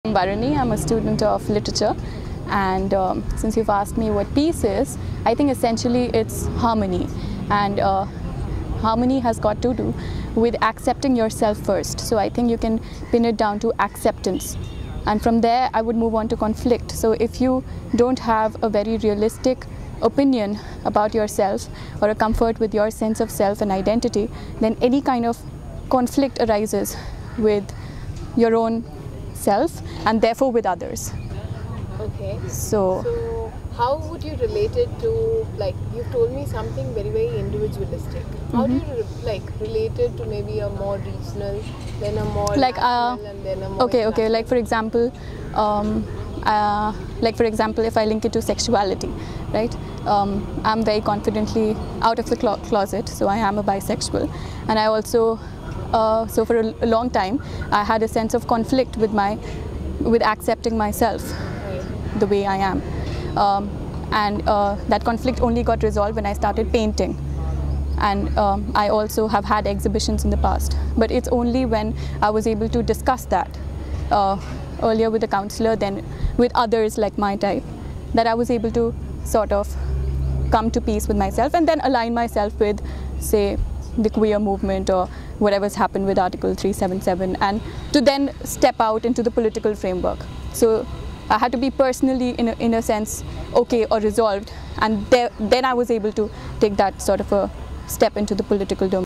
Sexuality and freedom.mpg
Baruni, a JNU student connects peace and sexuality in a way which was new for me.